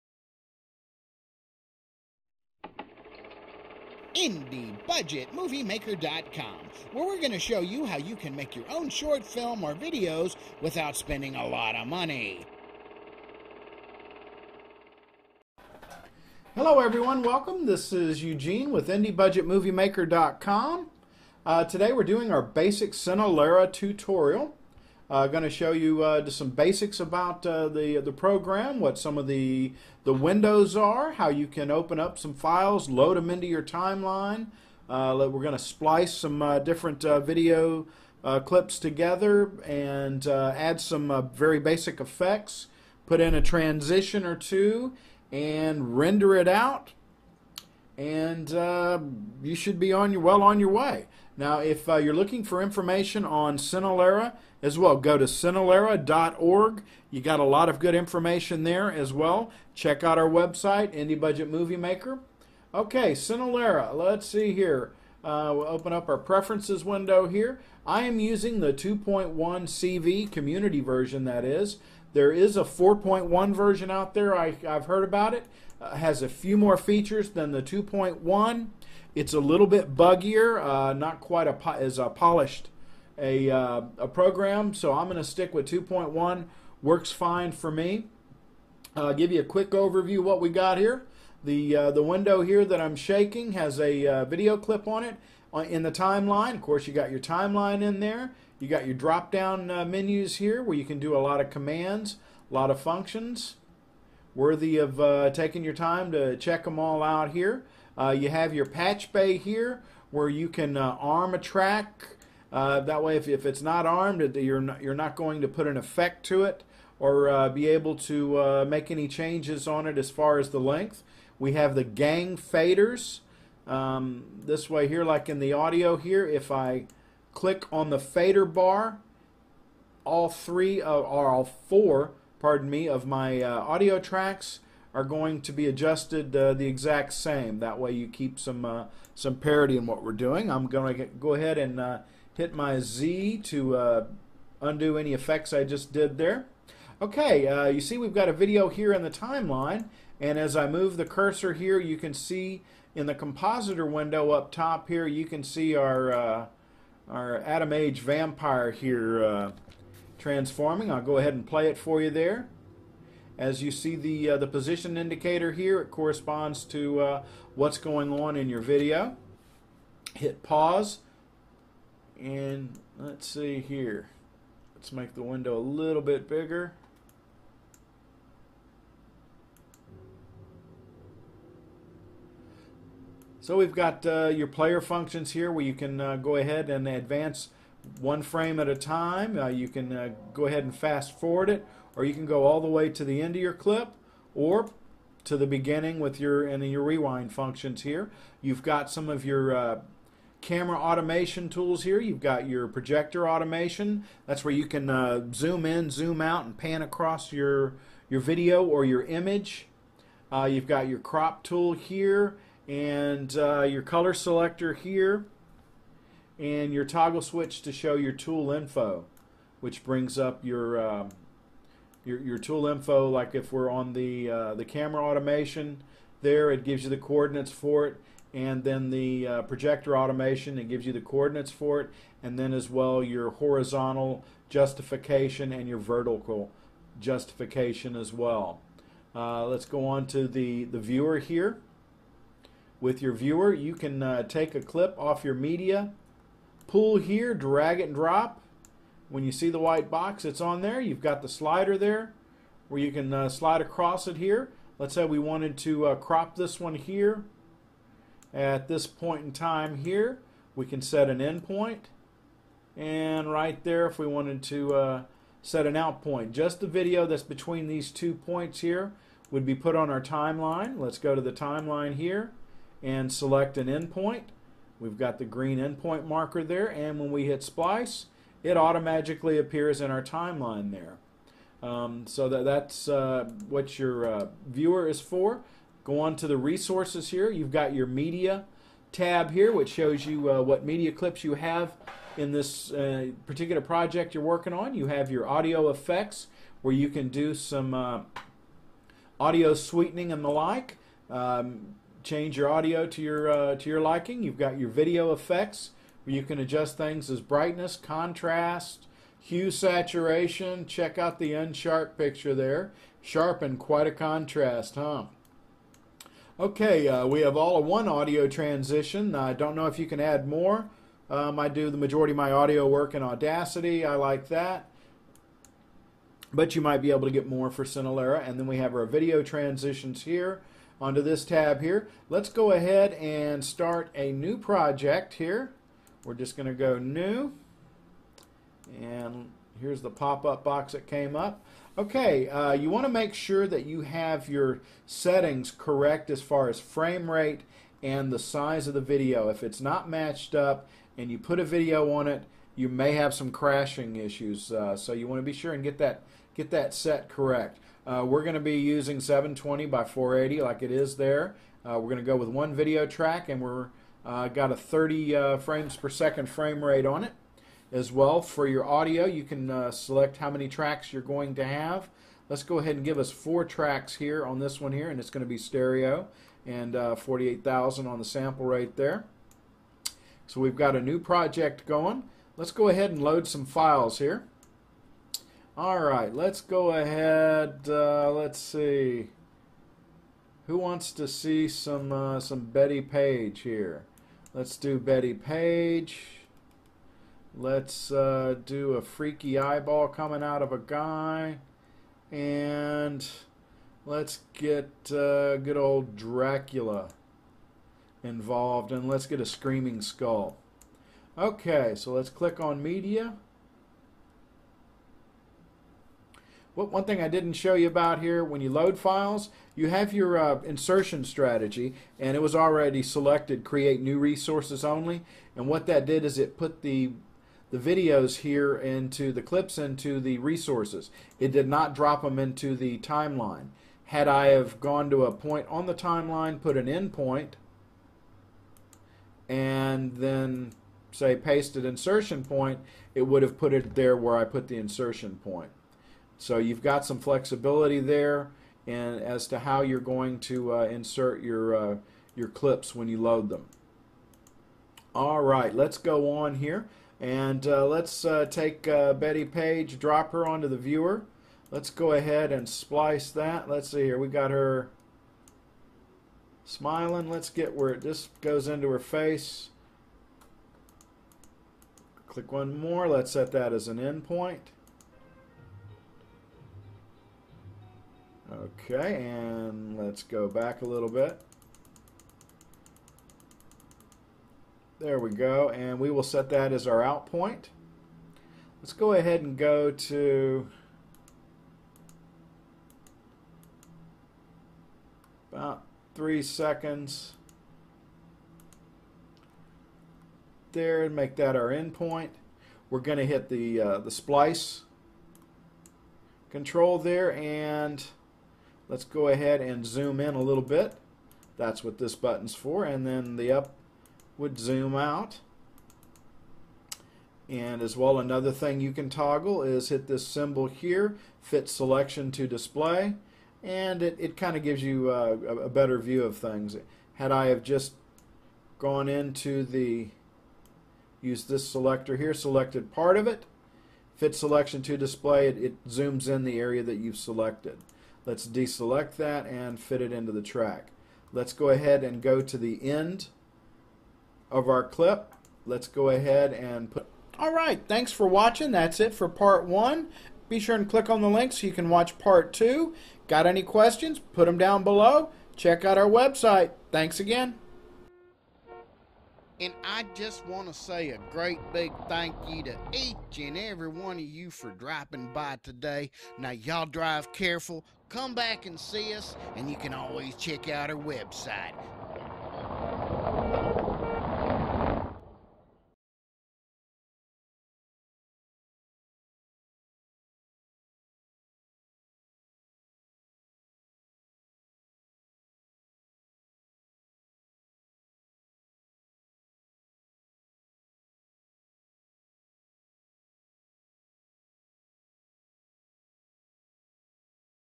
Part 1 Basic Cinelerra Tutorial - Free Video Editor for Linux
presents Basic Cinelerra Tutorial - Free Video Editor for Linux. You can use Ubuntu Linux to expertly edit HD Videos